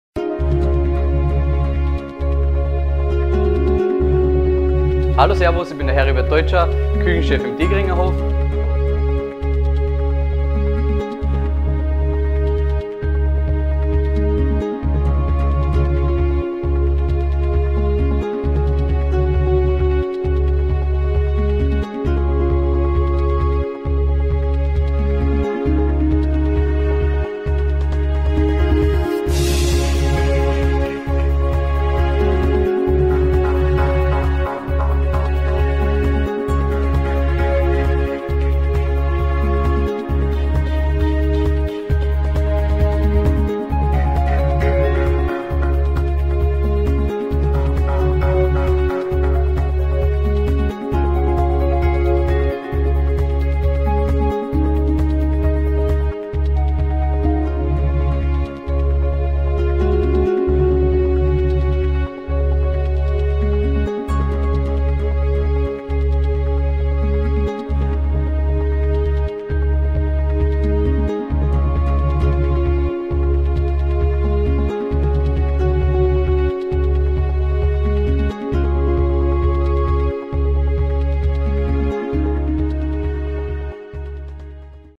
Demo Reel Rezeptvideo
5 Kameras (SLR) Canon 1D MK IV, 5D MK II, 7D, 600D, und Nikon D90
1871 Gigabytes an Daten
5 Drehtage 14 Rezeptvideos + Kinospot
4 Blickwinkel + Detail Shots
Multicam-Edit und Gradient in Final Cut